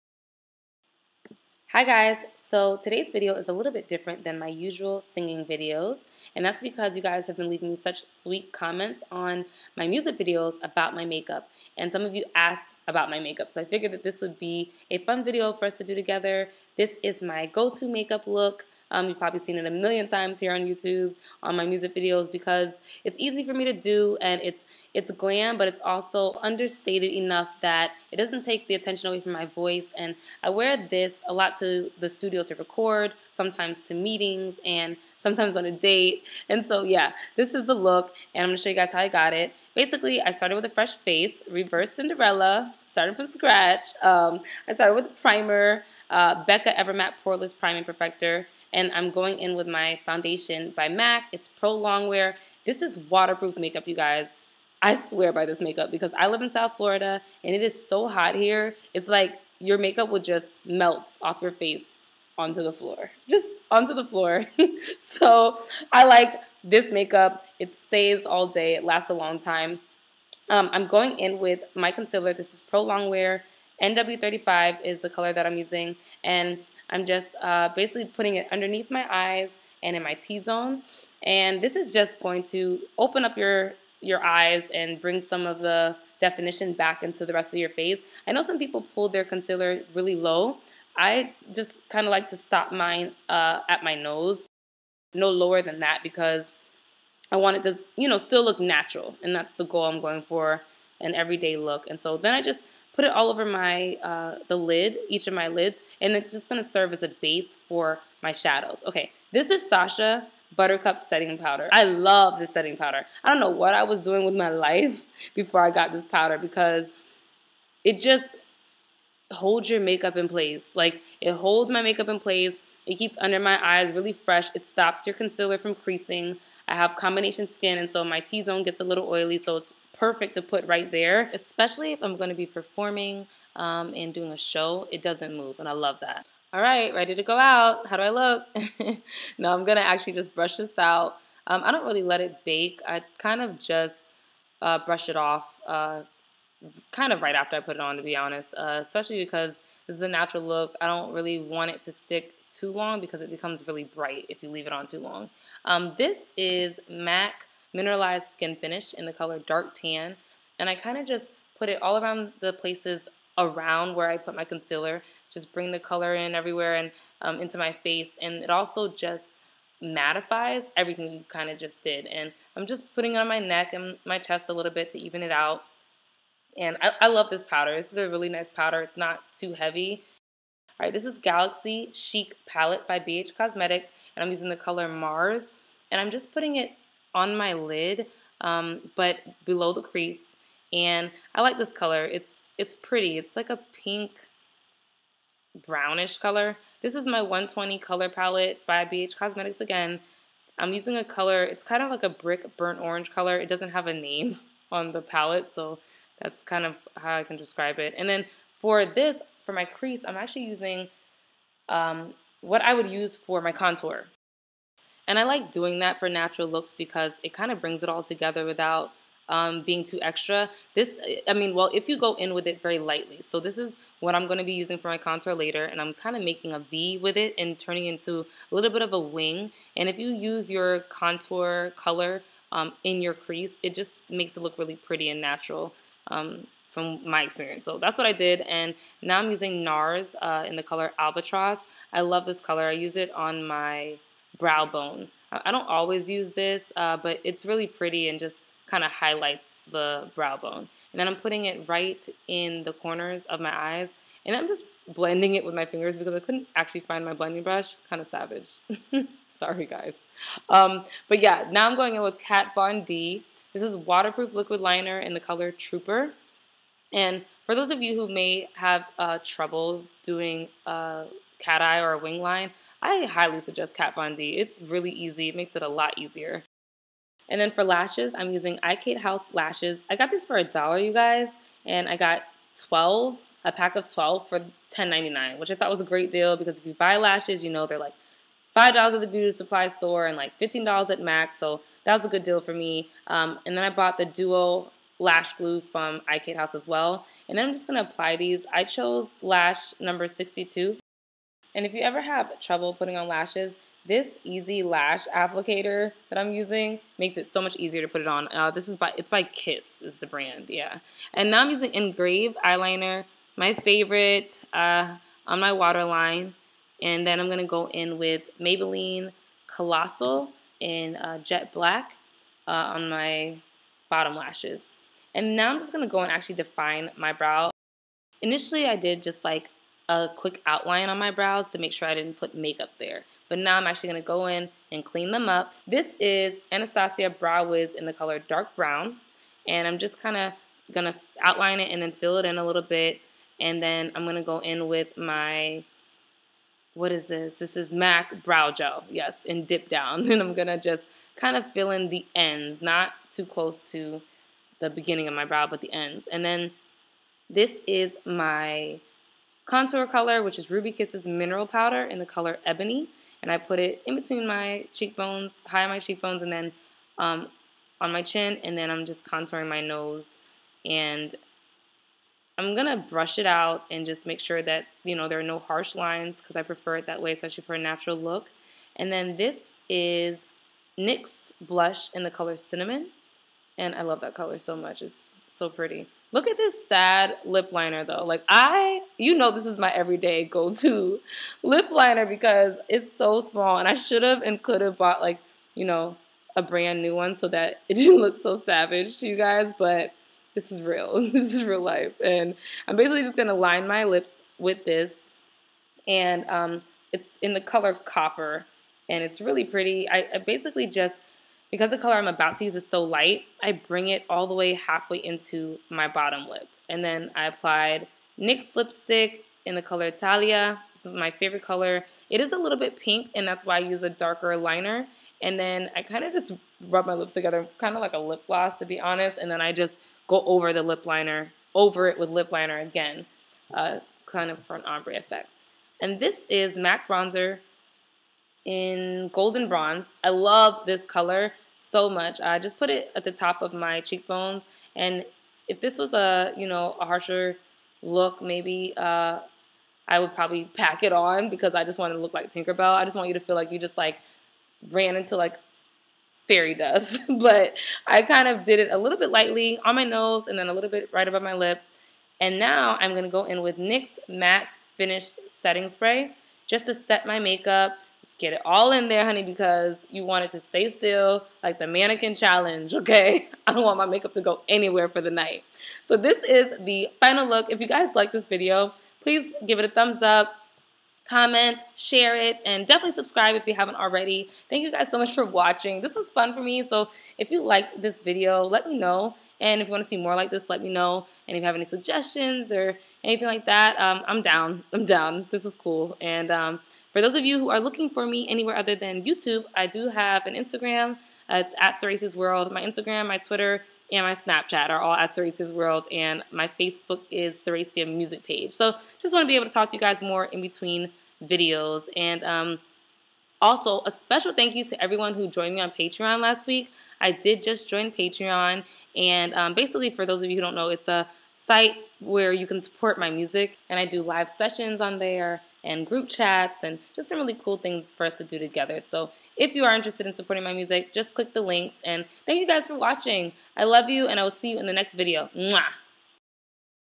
Everyday Makeup Tutorial
Thank you guys for watching! I had fun doing this video. If you liked it and want to see more like this just let me know k!

PRODUCTS USED:
Primer: Becca (Ever-matte poorless priming perfector)
Foundation: MAC Pro longwear Waterproof (NC42 and NC50         )
Concealer: Pro longwear NW 35
Sacha Buttercup setting powder
MAC mineralize skinfinish - Darktan
Galaxy Chic palette by BH cosmetics - color Mars
120 Palette by BH cosmetics - brick / burnt orange color
Ruby Kisses mineral powder - color Ebony
NARS - albatross
Kat Von D - waterproof liquid liner in Trooper
ikatehouse lashes #62
DUO lash glue from ikatehouse
eyelash easy applicator by KISS
Engrave liner by MAC
Collassal mascara by Maybelline
Anastasia brow wiz - Dark Brown
MAC brow gel - dip down
NYX blush in Cinnamon
NYX lip liner - Copper
NXY lipstick - Thalia
MAC bronzer - golden bronze
NYX Matte Finish Setting Spray

★☆★ FIND ME! ★☆★